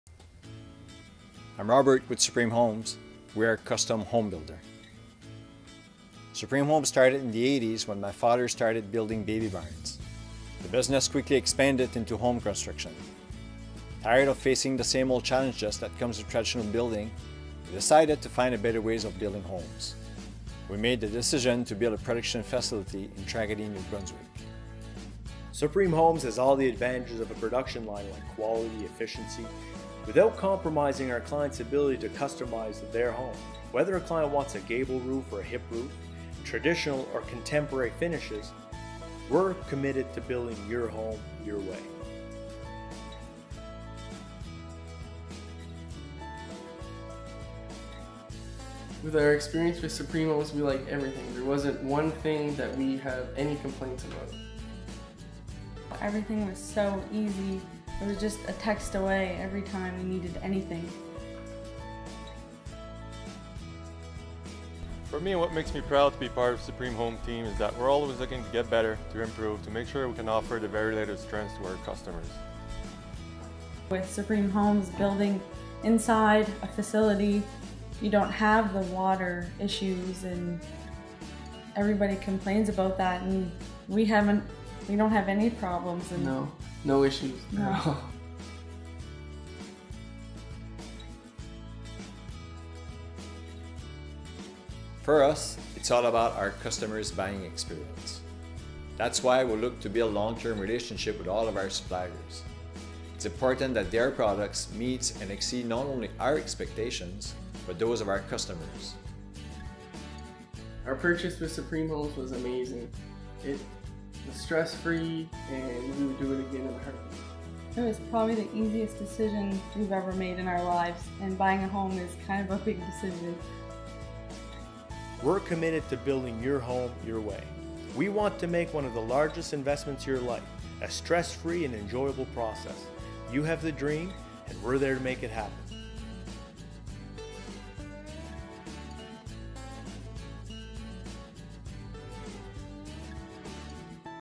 Canadian Modular Home Builders | Supreme Homes
Supreme Homes’ houses are built to the highest Canadian standards under climate controlled conditions.  Our uncompromising dedication to excellence and the stringent quality control in our plant make us the best home builder on the market.